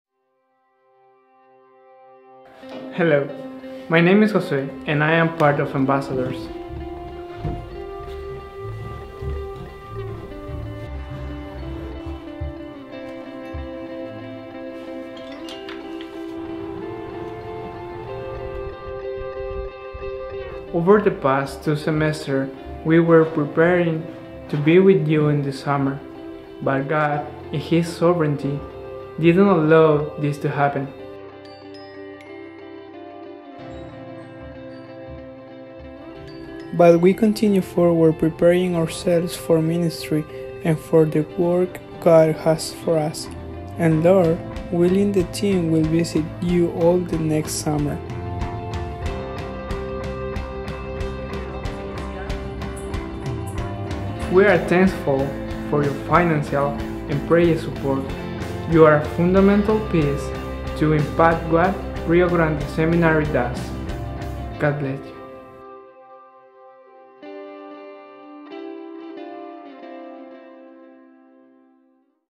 A message from The Ambassadors - Rio Grande Bible Ministries
The Ambassadors are a music team of student from the bible college at Rio Grande. Normally they travel and sharing with churches and ministries across the country but in God sovereignty they were unable to this year due to the coronavirus. We continue on in the ministry onward in the ministry that the Lord has given us and we look forward to seeing you next year!

Rio Grande Bible Ministries - Seminario Bíblico Río Grande